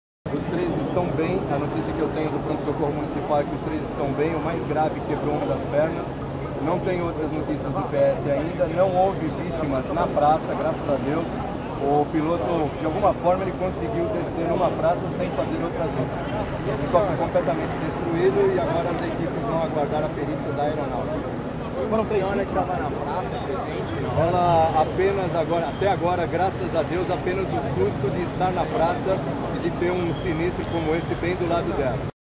SECRETÁRIO DE SEGURANÇA FALA SOBRE QUEDA DE HELICÓPTERO
José Quesada Farina com as primeiras informações após a queda de uma aeronave no Centro de São Caetano.